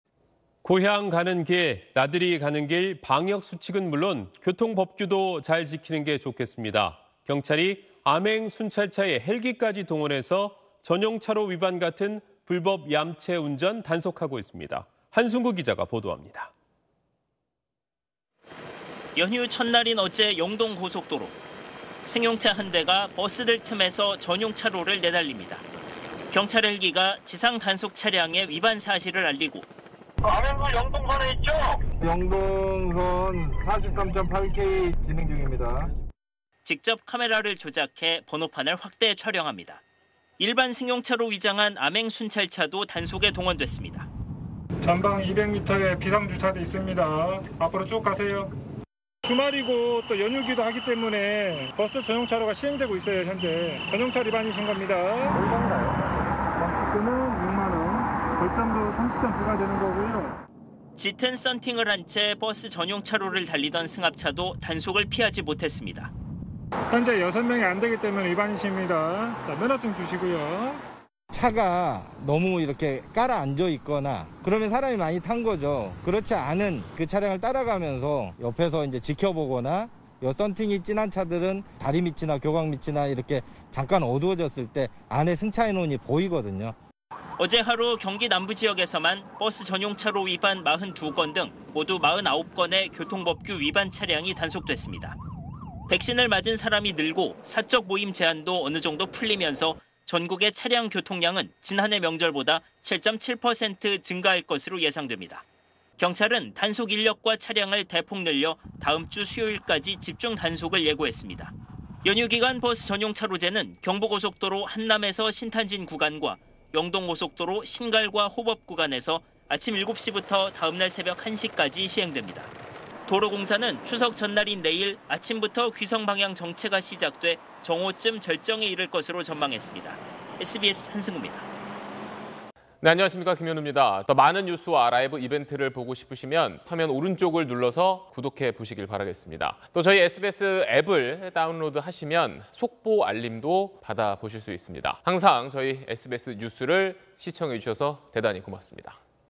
귀성 · 귀경길 '얌체족' 꼼짝 마…위반 차량 49건 단속 / SBS
고향 가는 길, 나들이 가는 길, 방역 수칙은 물론 교통 법규도 잘 지키는 게 좋겠습니다. 경찰이 암행 순찰차에 헬기까지 동원해서 전용차로 위반 같은 불법 얌체 운전 단속하고 있습니다.
한승구 기자가 보도합니다.